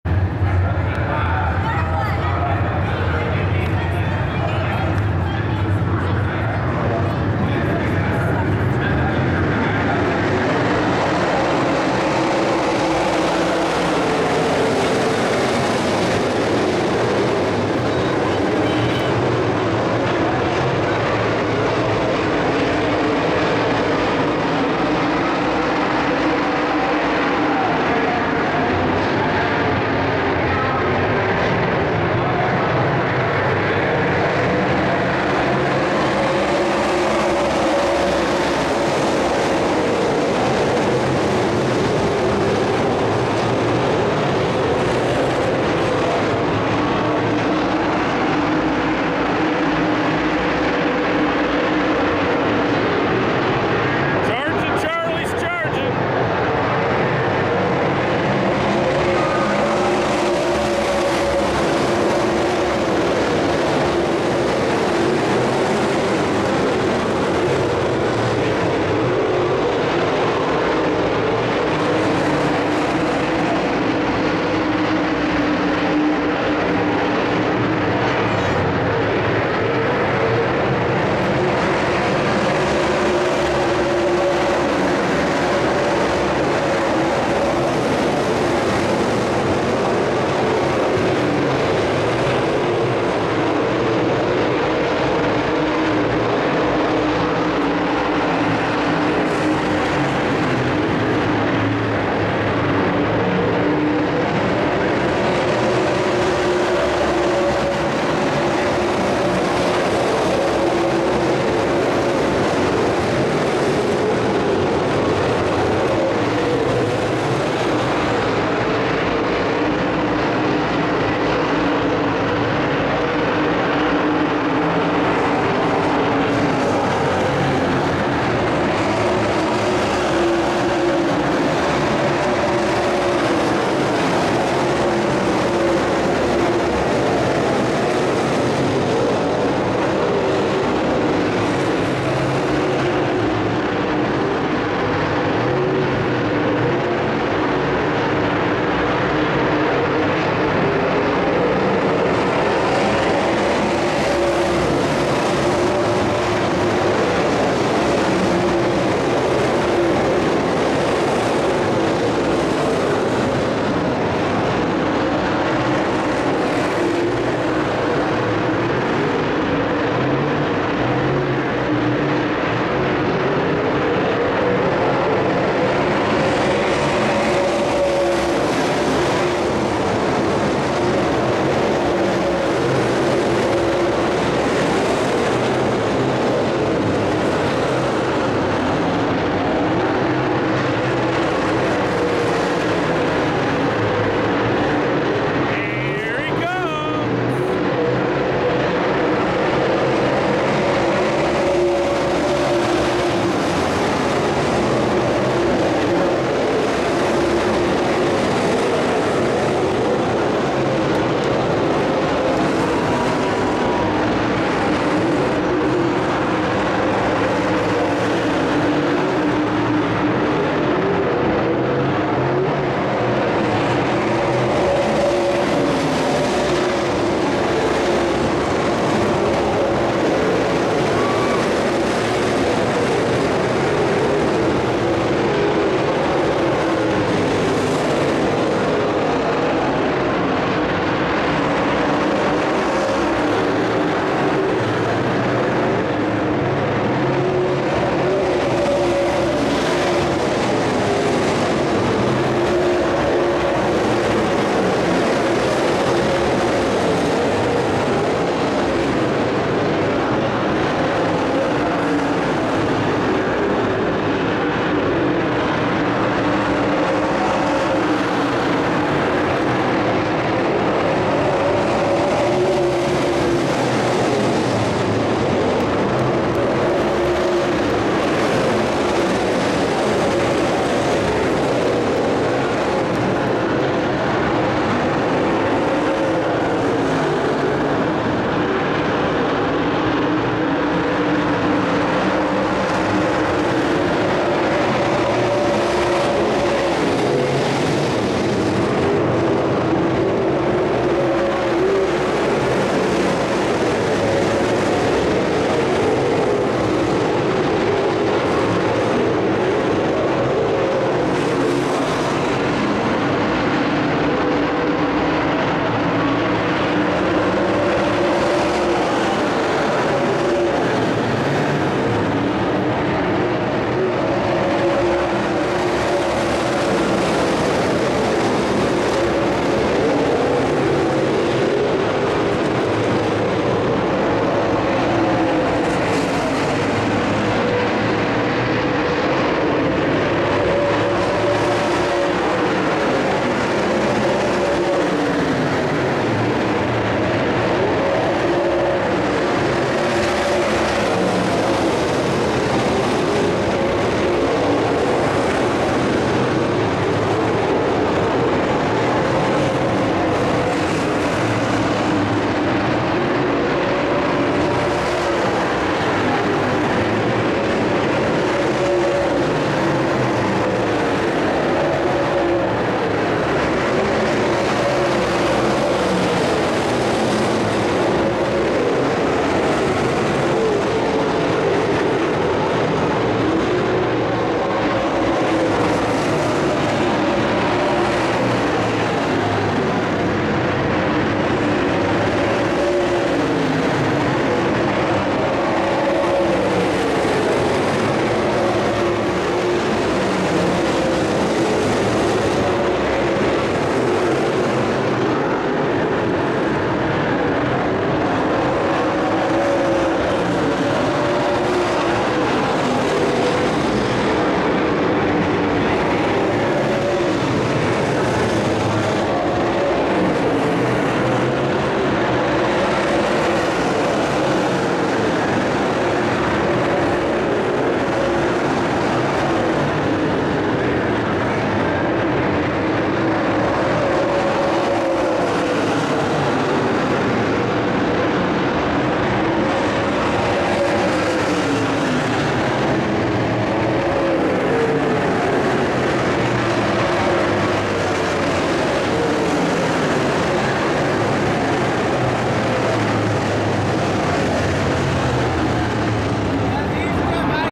ISFS LM A 5-22-15
ISFS Late Model A Main won by 77 Jeff Aikey in Des Moines Iowa.